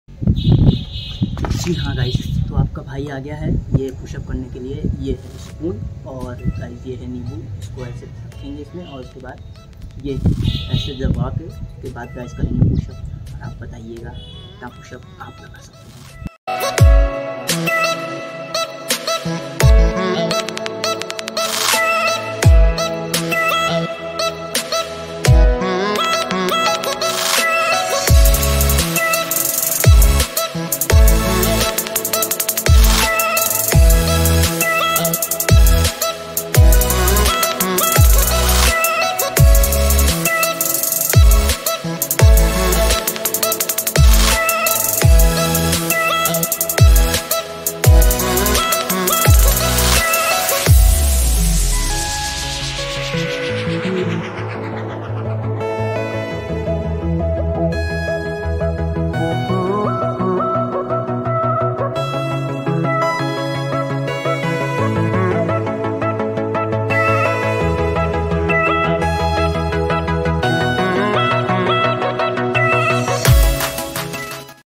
Lemon Challenge 🍋 💪 || Day 59
So guys, this video is for how to build your body without going to gym.

Home workout is the best workout.💪

Guys agar aap log bhi Bina Paisa khrch kiye apni body ko fit rakhna chahte hai to Pushup jrur lagaye isse apki kaam krne ki shakti bdhegi or app khush rhnge.😇😇